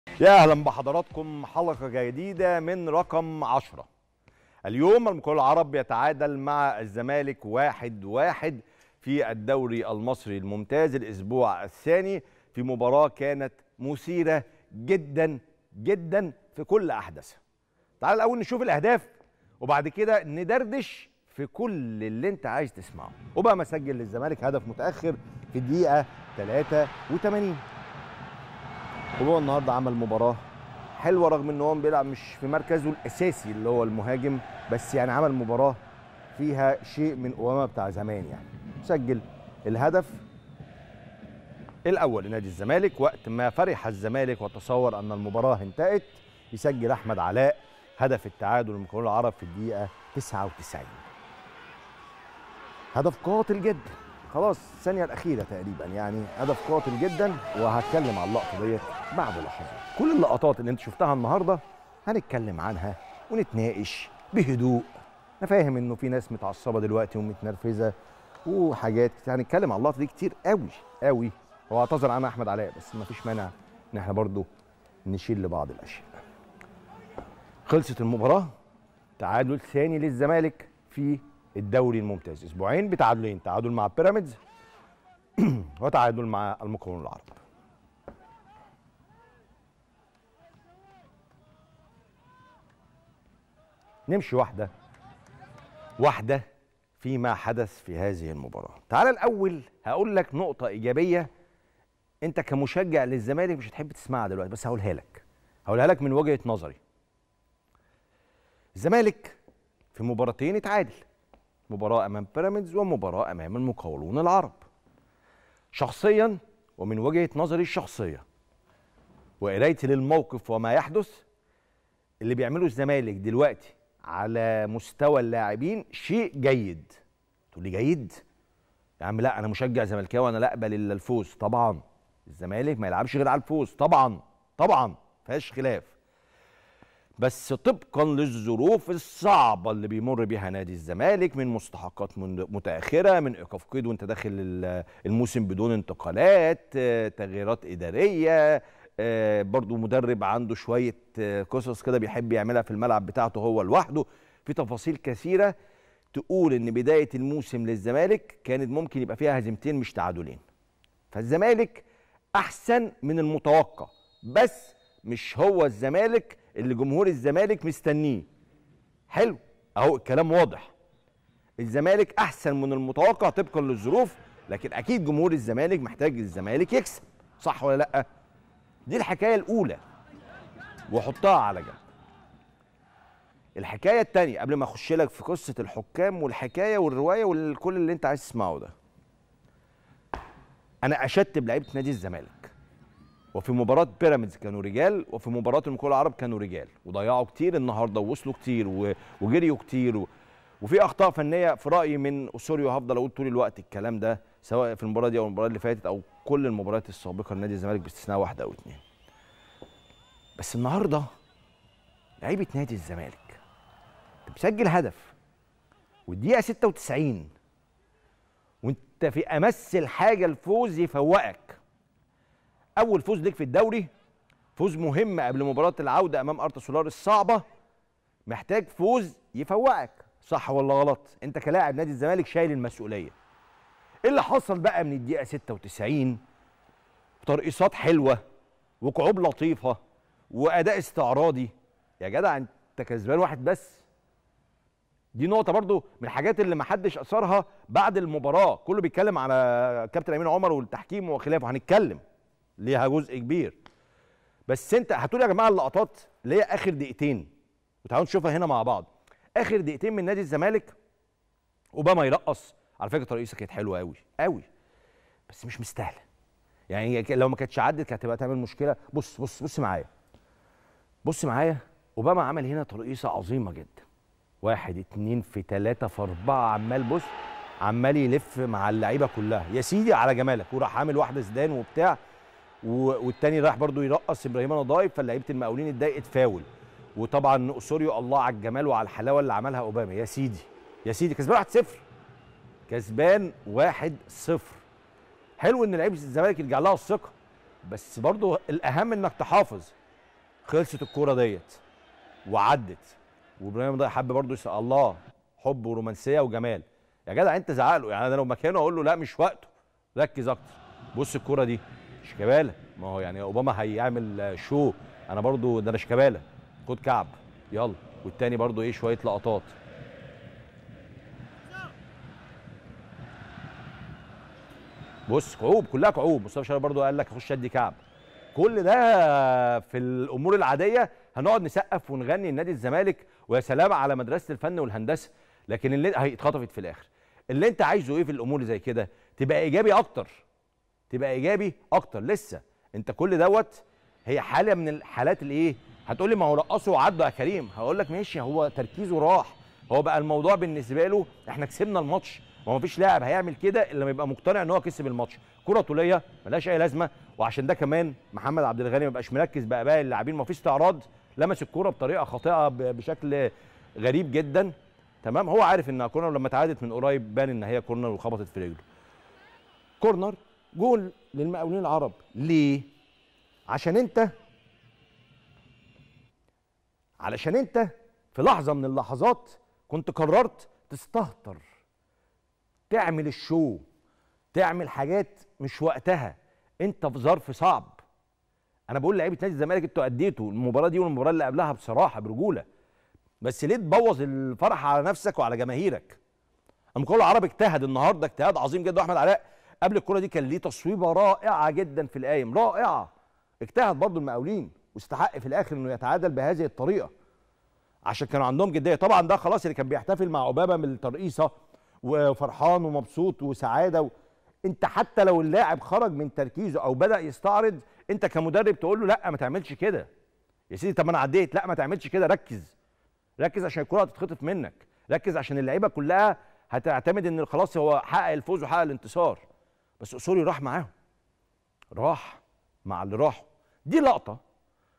"الدنيا مش مستحملة" تعليق ناري من كريم رمزي على الحالة الجدلية في مباراة الزمالك والمقاولون🔥🔥
🔥🔥"ليه بنبوظها؟! .. الدنيا مش مستحملة" تعليق نااااااري من كريم رمزي على الحالة الجدلية في مباراة الزمالك والمقاولون🔥🔥